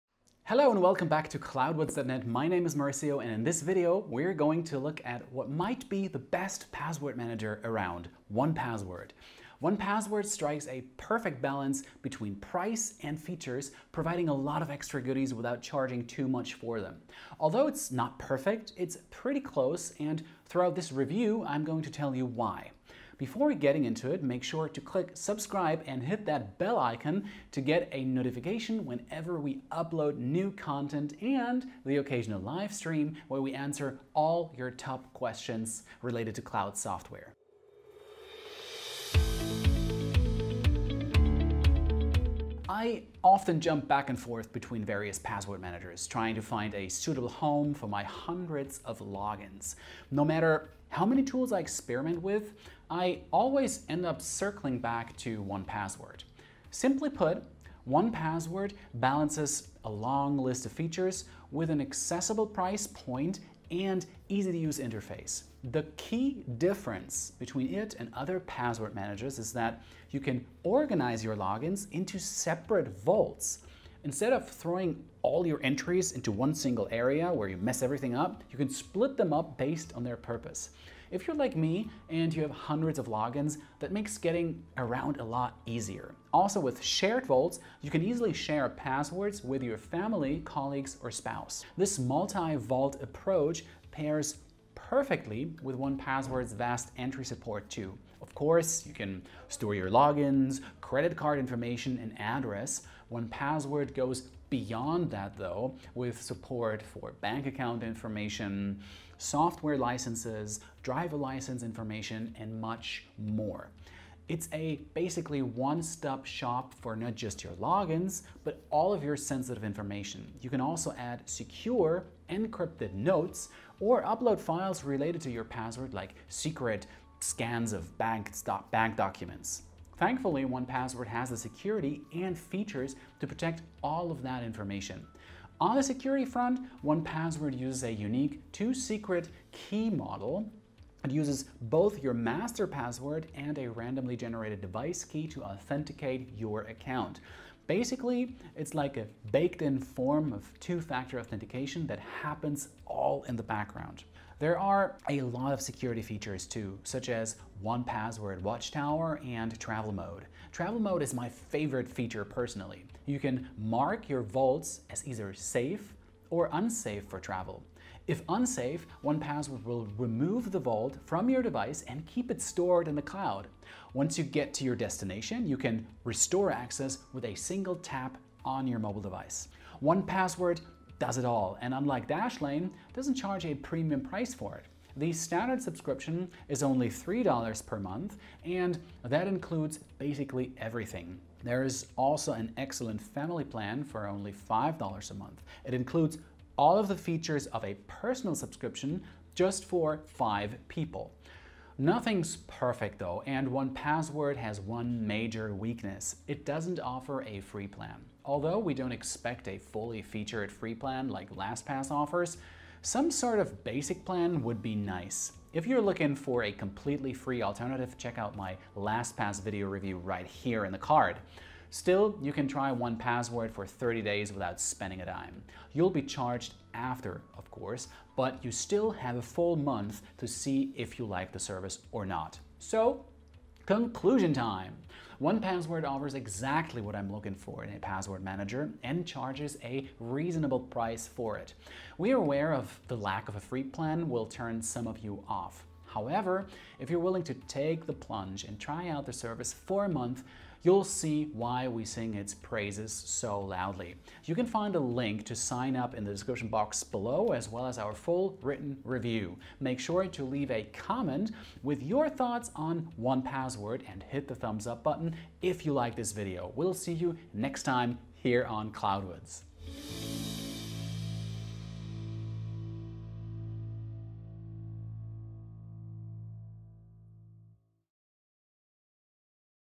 1Password Review: Can it be the one?
0:42 1Password
1:05 Separate vaults
2:00 Secure encrypted notes
2:15 Two-secret key security model
2:34 Watchtower and Travel mode
2:59 Price
3:21 No free plan
3:52 Conclusion

1Password has always been a favorite here at Cloudwards.net. Even with rising prices at Dashlane and LastPass, 1Password charges the same rate, and includes a long list of features at that price. Watch our full 1Password review to see why we rank it as the best password manager.